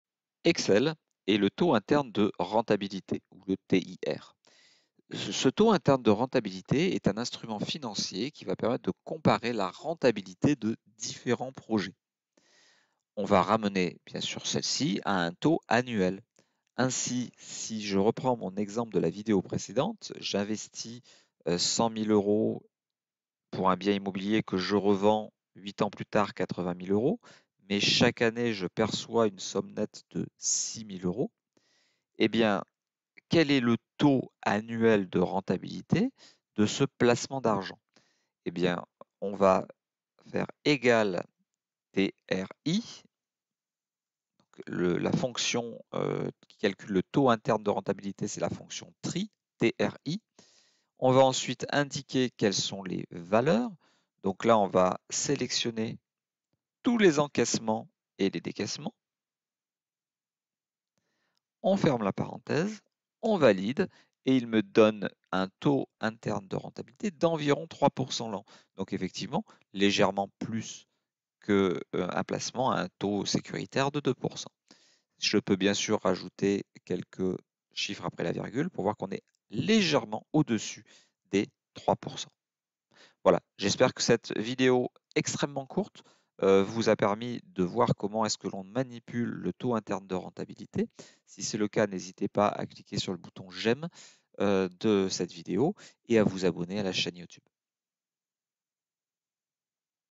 XL05 - 044 - Le Taux Interne de Rentabilité
Comment calculer le Taux Interne de Rentabilité avec Excel ?